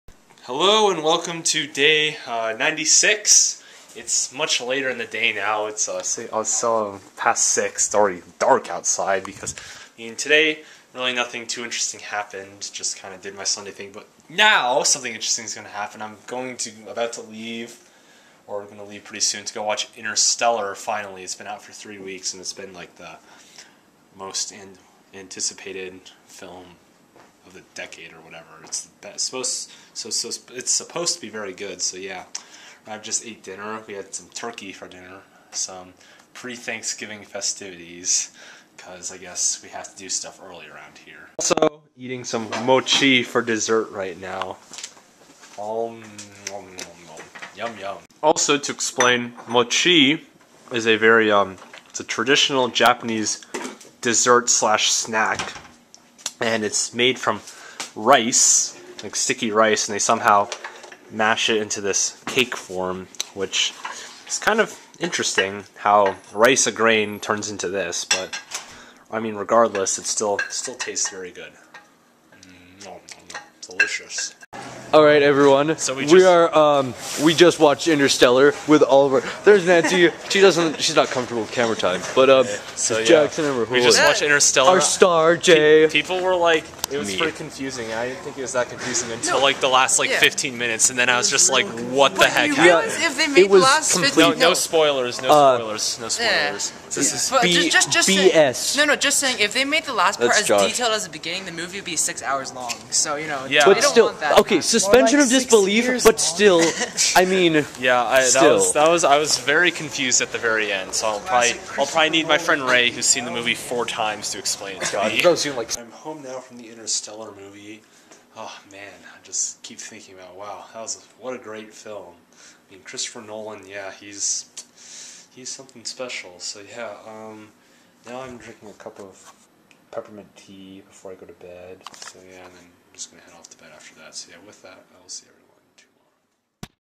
Interstellar! - (Day 96 - November 23rd, 2014)
Today I started the vlog a little later than usual. I ate some mochi for dessert and then watched the new Christopher Nolan film Interstellar with my friends! I thought it was an amazing movie and Nolan is definitely going to go down in history as one of the best directors to ever live.